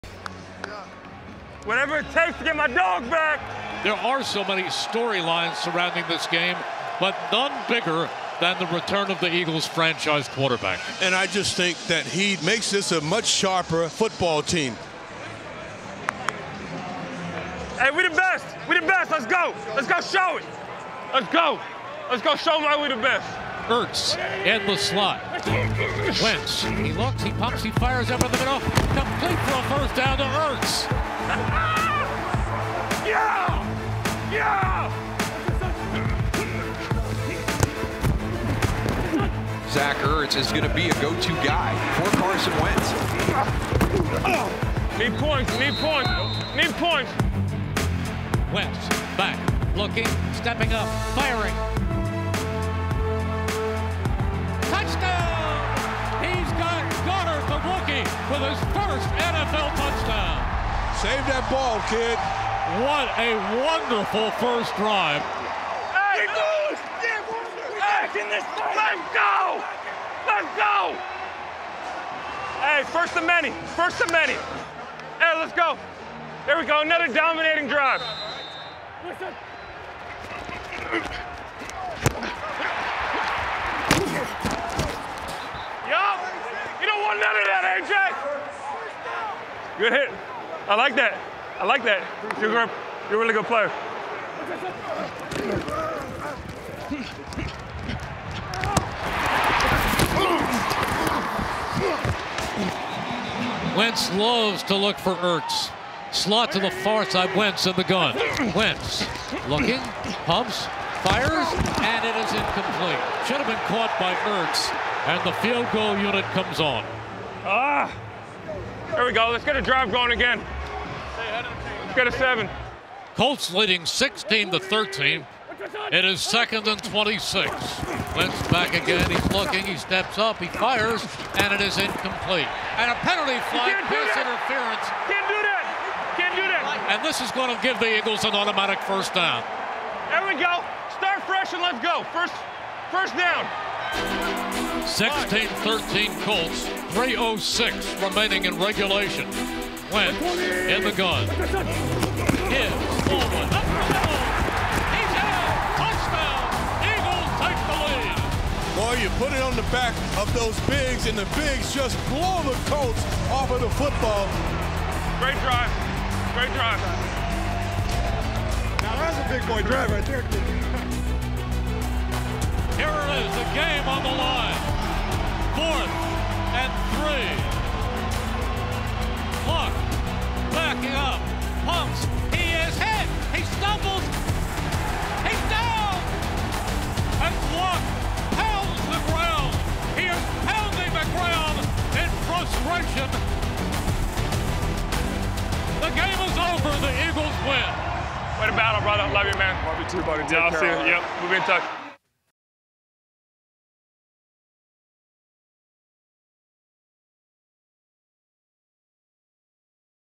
Zach Ertz Mic'd Up vs. Colts "I Like That Hit, You a Really Good Player!" | NFL Films
Listen to Philadelphia Eagles tight end, Zach Ertz, mic'd up against the Indianapolis Colts during Week 3.